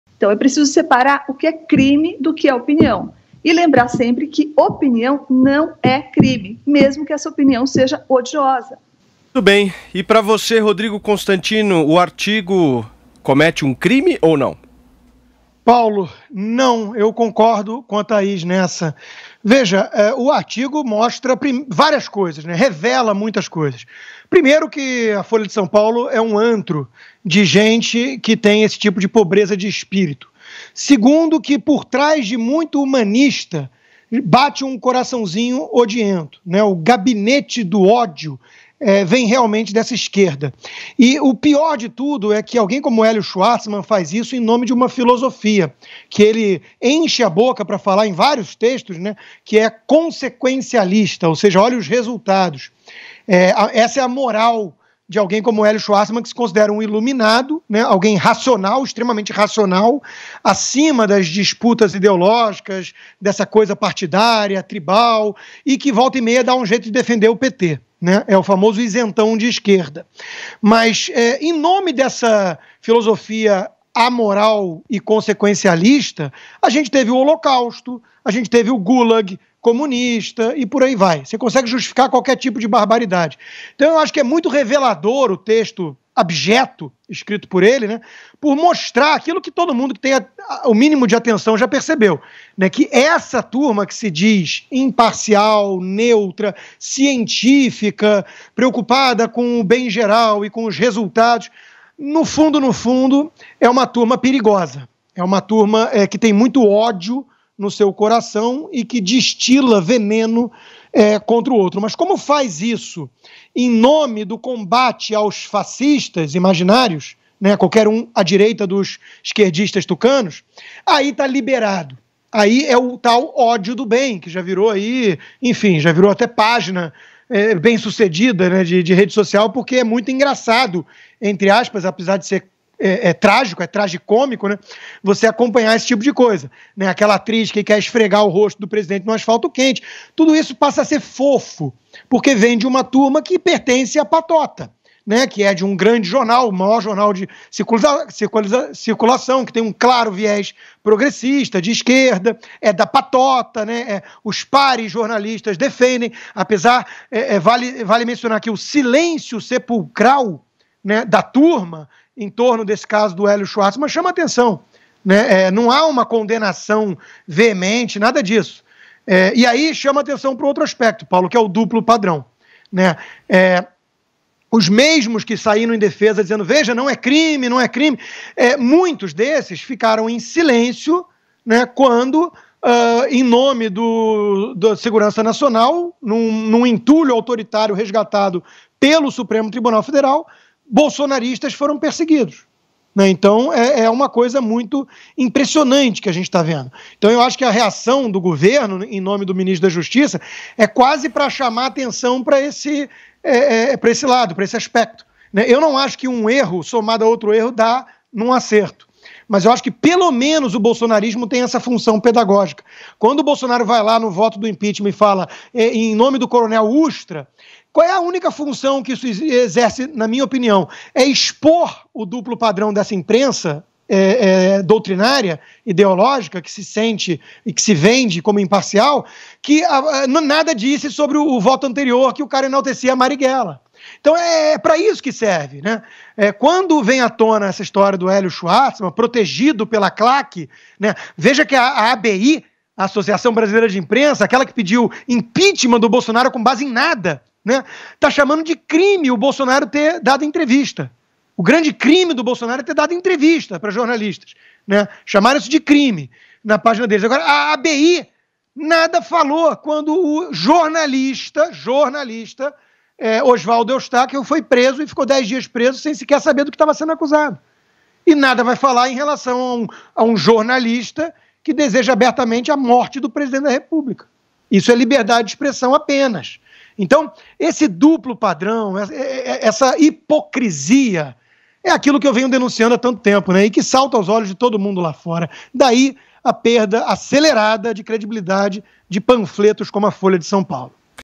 O DUPLO PADRÃO DA EXTREMA IMPRENSA
Mordaça e prisão para quem criticar o STF.
Liberdade para caluniar, agredir e apoiar a morte do Presidente.